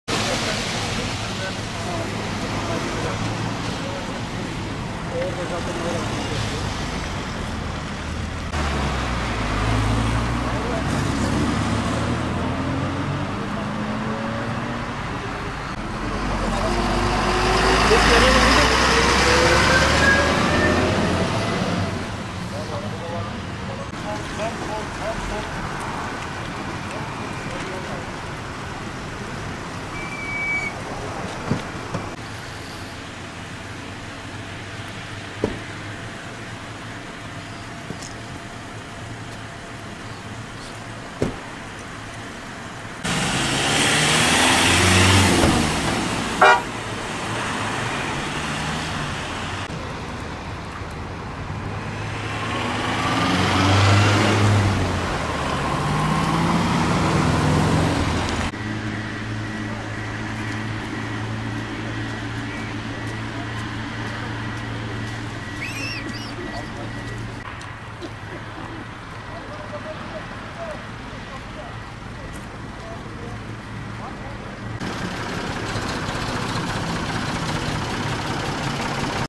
Hastaneye giderken hastanelik oldular
Bolu’da, sisin etkili olduğu D100 Karayolu’nda 2 otomobilin çarpışması neticesi meydana gelen kazada tedavileri için hastaneye giden 2 kişi yaralandı.